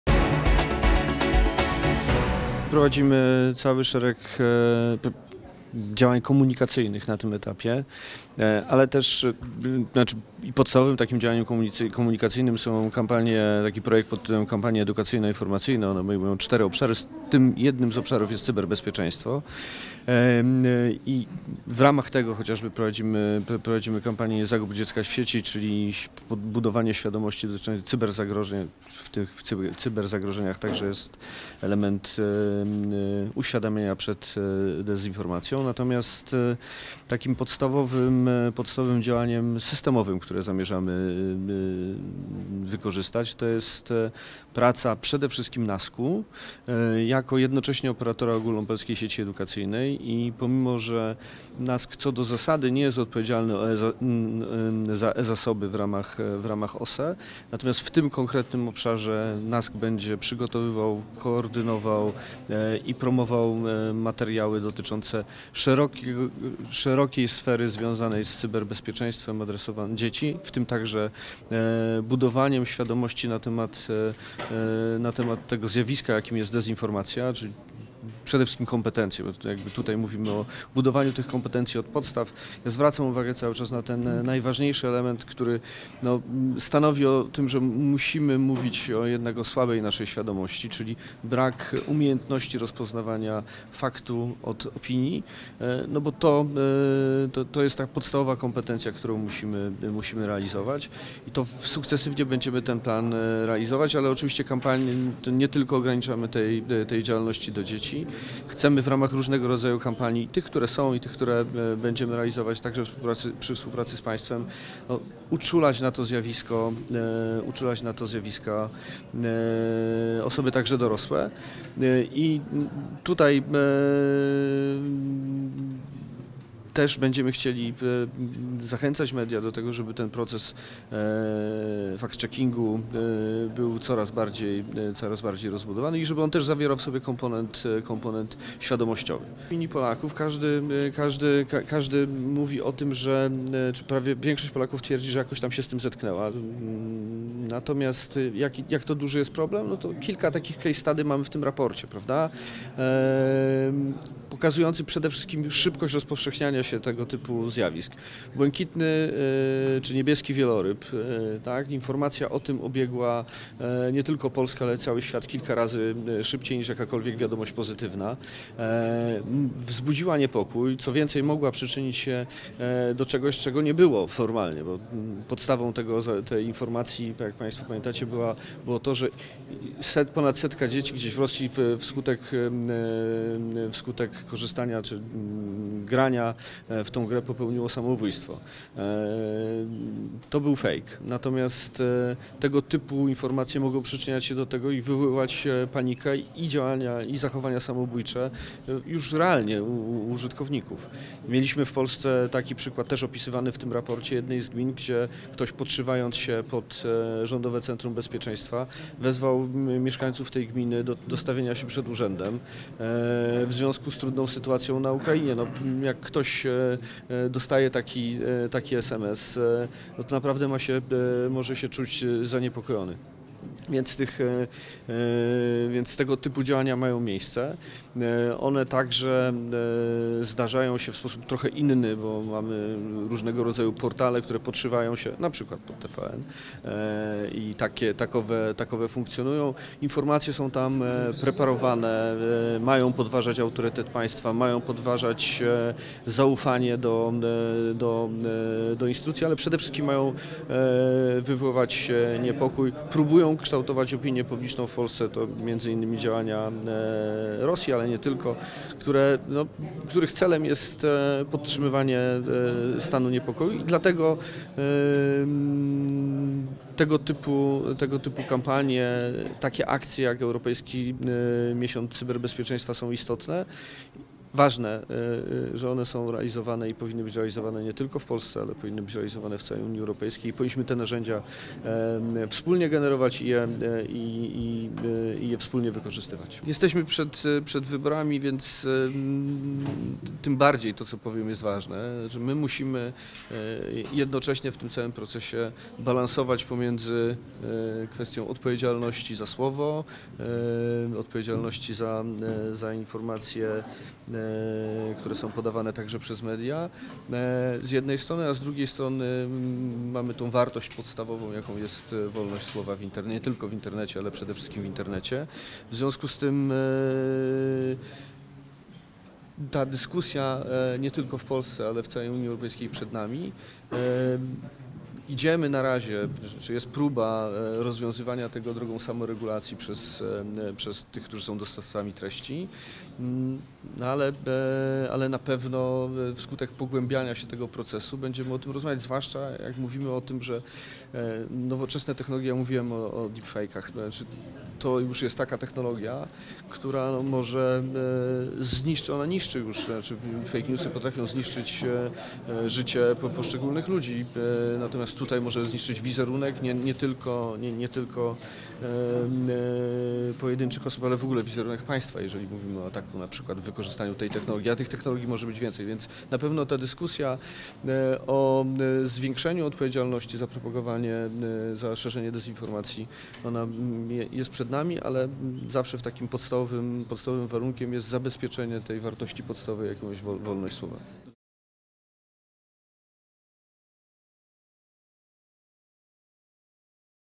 Minister cyfryzacji : edukacja kluczem do walki z dezinformacją
O znaczeniu edukacji i działań komunikacyjnych w walce z dezinformacją, doświadczeniach Polaków z propagandą w sieci oraz zagrożeniu ze strony nowoczesnych technologii mówi minister cyfryzacji Marek Zagórski